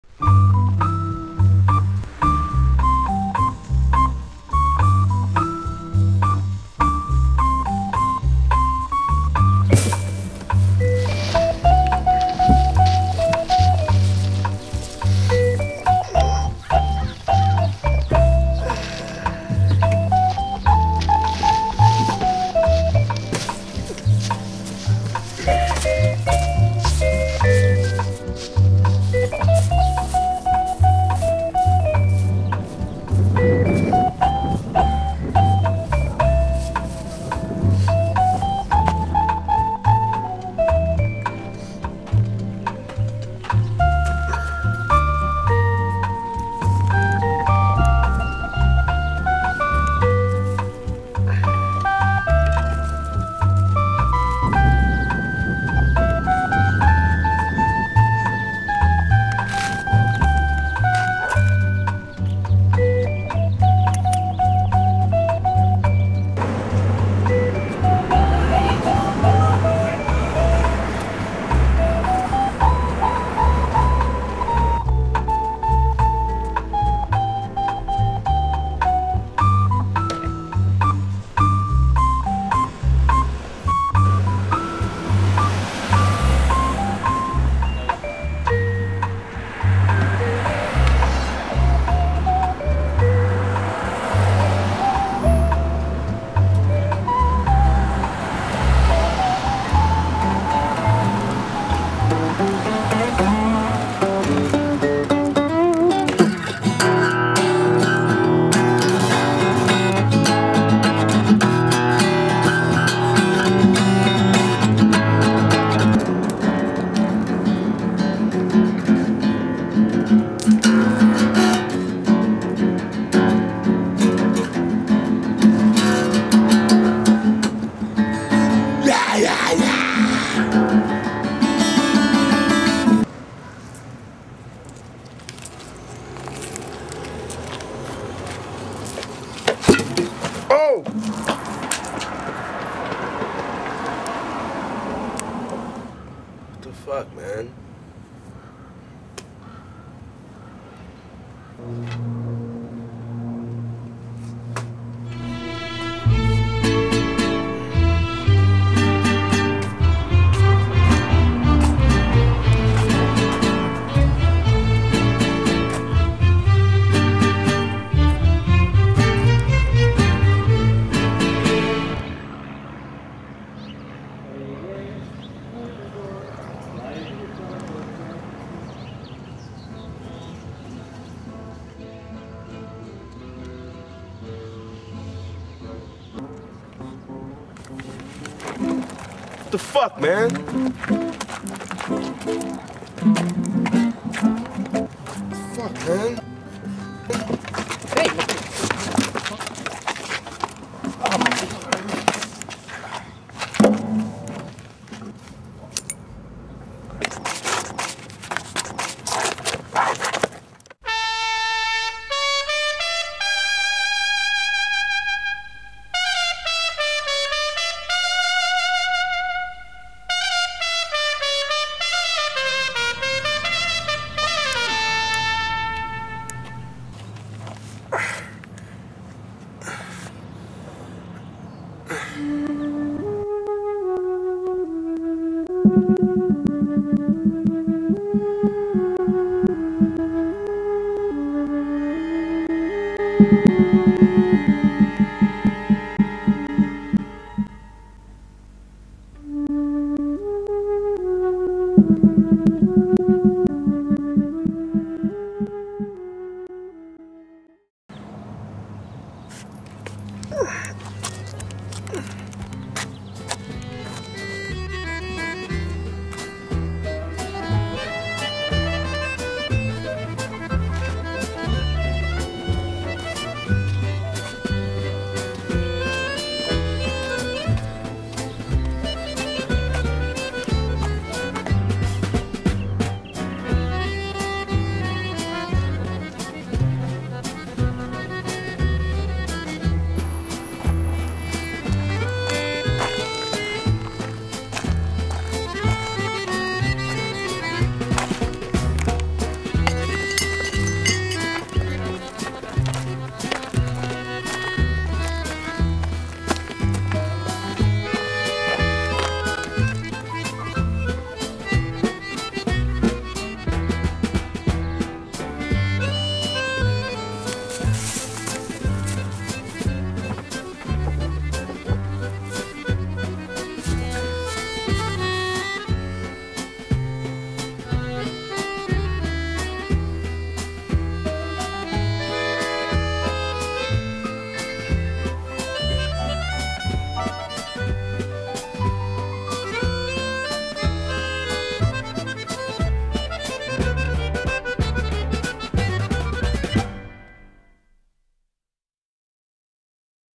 Bum Film
Short Film for class. Directed & Edited by Michael Maese. Story by Michael Maese & Will Mill. May 14th, 2009.